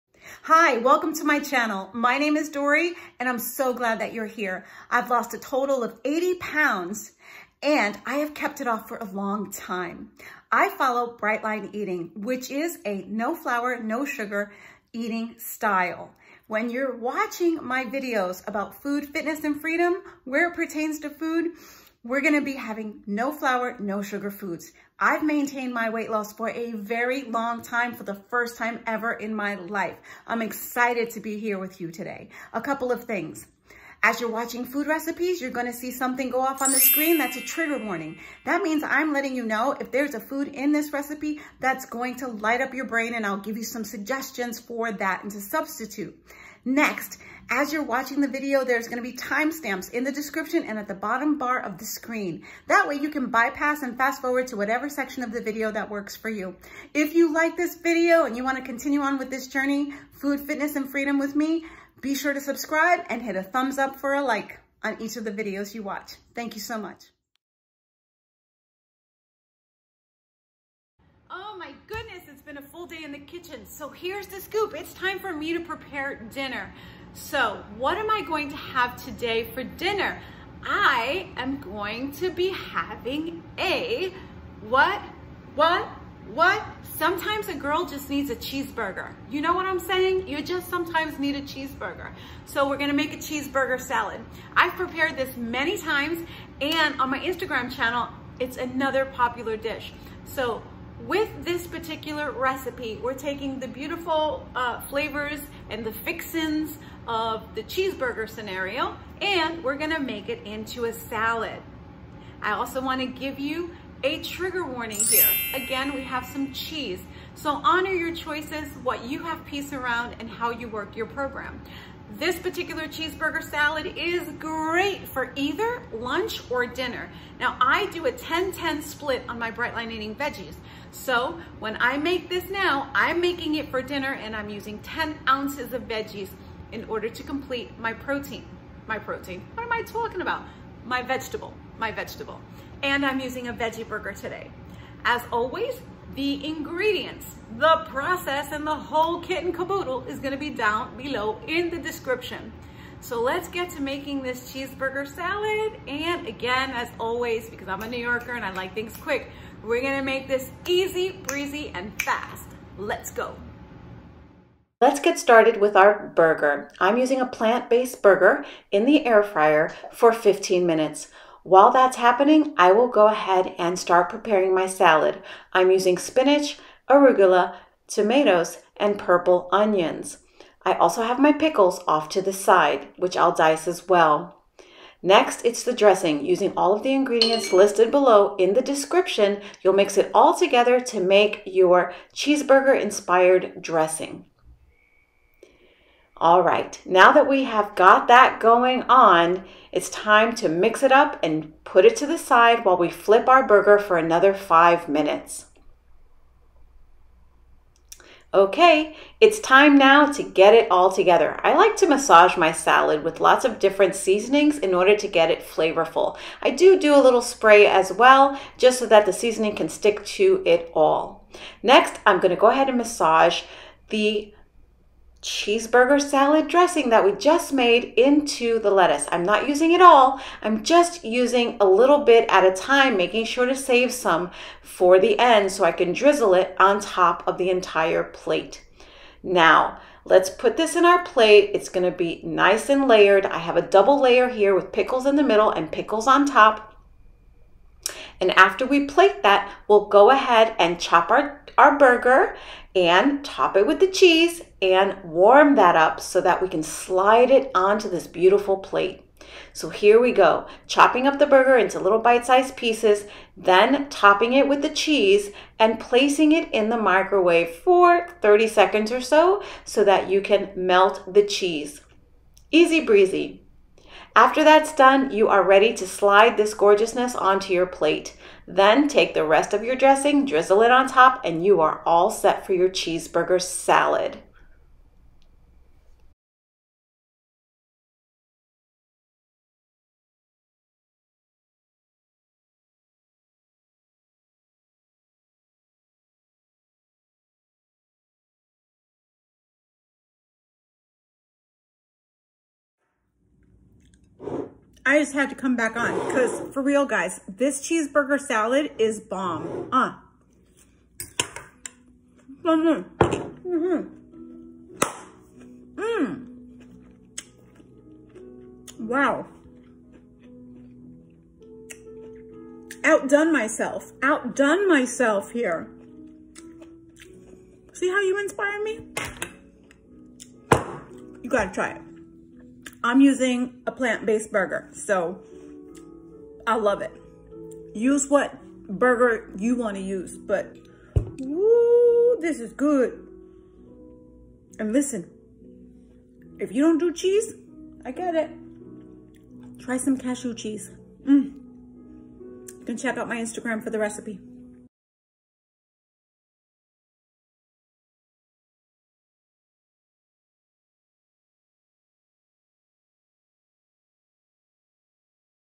COMFORT FOOD | Cheeseburger Salad
This hearty salad, with all the fixins’, hits the spot EVERY.SINGLE.TIME.

🔗 Tag me on IG if you make it!
INGREDIENTS
🔸3 oz Cooked Veggie Burger (or Other Burger)
🔸1 oz Shredded Cheddar Cheese (or Other Cheese You Like)
🔸10 oz Veggies like Lettuce, Tomato, Red Onions
🔸.5 oz Mayo

DRESSING
🔸EVOO or Avocado Oil Spray
🔸Heavy Squirt Mustard!
🔸Splash of Liquid Smoke
🔸Horseradish Sauce to Taste
🔸2 oz of Dill Pickles chopped into Salad
🔸Dill Pickle Juice from the Jar!
🔸Garlic, Salt, Pepper, EBTB Seasoning, Dill
🔸Water
TRIGGER WARNING
Cheese...remember that cheese has addictive qualities, so whatever you do, make sure you have peace around your choices.
TIMESTAMPS:
00:00 Intro
01:10 Recipe Info
01:54 Trigger Warning
02:44 Start the Recipe
03:04 Make the Dressing
03:27 Dress the Salad
04:00 Plate the Salad
04:19 Create the “Cheeseburger”
04:35 Top the Salad with the Cheeseburger
05:03 Let’s Taste It!
BRIGHT LINE EATING NOTES
►BLE = 1 Protein, 1 Fat, 10 oz Veggie
►The above recipe is based on how I follow BLE portions for a Woman. This is my complete dinner when I follow the Weight Loss model of Bright Line Eating with the 10/10 Veggie split. On Maintenance or for Athletic additions, I would add an extra grain on the side.
►For Bright Line Eaters, this is a versatile salad for lunch or dinner!
KITCHEN TOOLS
Cutting Board Scissors